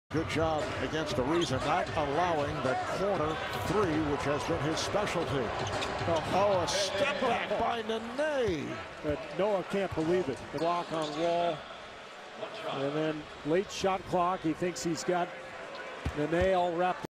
NBA, playoff 2014, Bulls vs. Wizards, Round 1, Game 5, Move 33, Nene Hilario, 2 pointer
Chicago Bulls - Washington Wizards, Game 5, 29/04/2014, quarter 3
Nene Hilario, 2 pointer